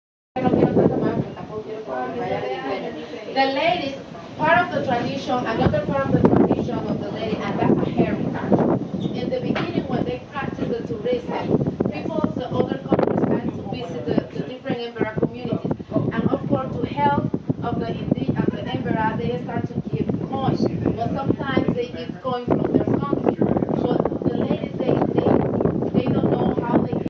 View from Gamboa tower of lake and Panama Canal
Sweeping view of the area around the Gamboa overlook tower with the lake and part of the Panama Canal.